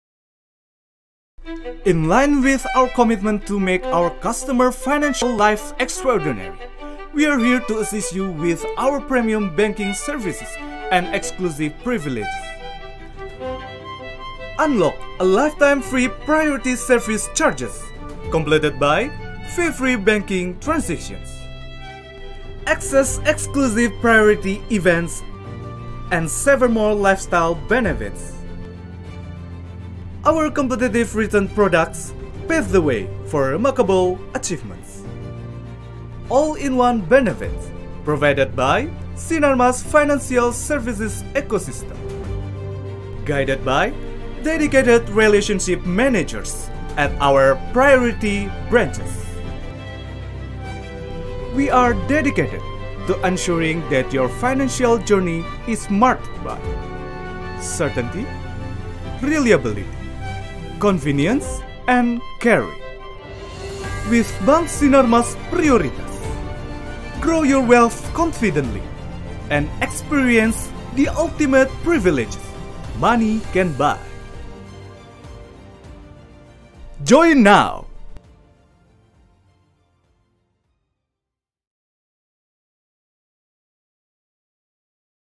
Bank Sinarmas Prioritas!
Dengan bangga kami persembahkan layanan Bank Sinarmas Prioritas sebagai wujud komitmen kami untuk memberikan pengalaman perbankan yang istimewa untuk Anda. Nikmati berbagai fasilitas eksklusif dan layanan terintegrasi yang dirancang khusus untuk perencanaan finansial Anda.

Bersama Bank Sinarmas Prioritas, perjalanan untuk mencapai tujuan finansial Anda jadi luar biasa.

Bergabunglah sekarang dan RayakanPencapaian!